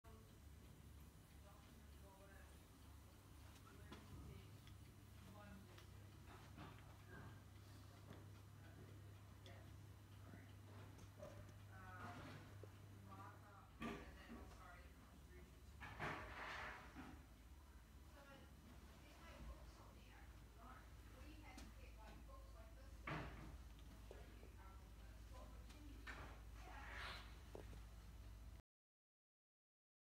Baldivis His & Hers
A great walk-in Wardrobe Fit out with all the storage you could ever need a place for everything and everything in its place